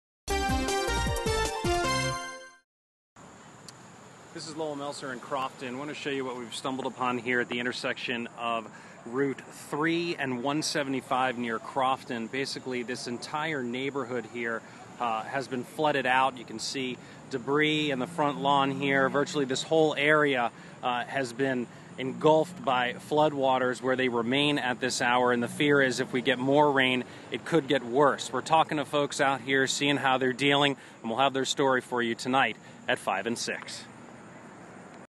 Lowell: Debris Floating On Flood Waters In Crofton
Residents in Crofton are concerned about rising flood waters carrying debris precariously close to their homes.